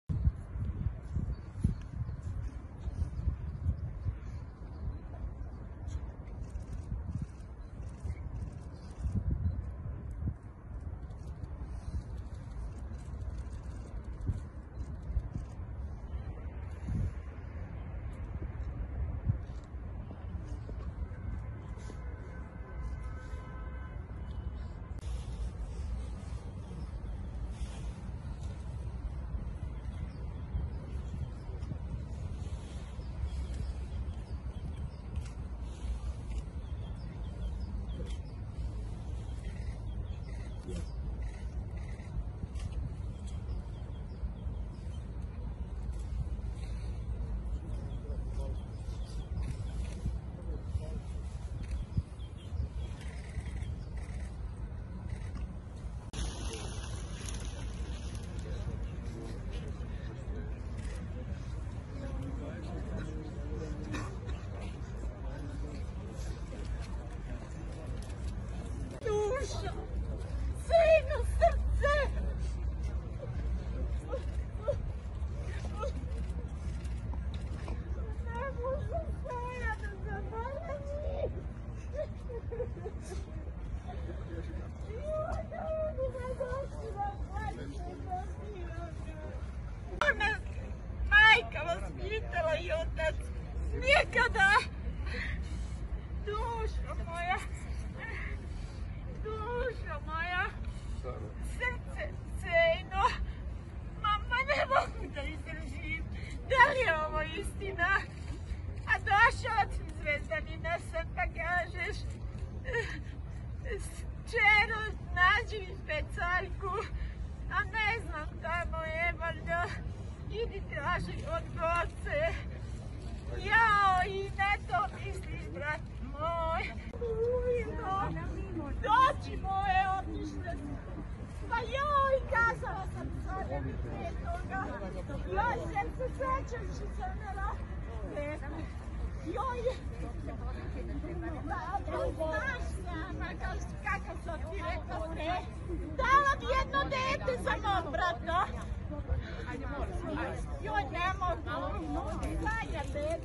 POTRESNO, ŠABANOVA SESTRA NARIČE I JECA: Seja ne može da te zaboravi! Goca je smirivala!
Pevač Šaban Šaulić, kralj narodne muzike, izgubio je život u saobraćajnoj nesreći pre 3 godine. Danas je održan trogodišnji pomen, na Novom groblju u Beogradu.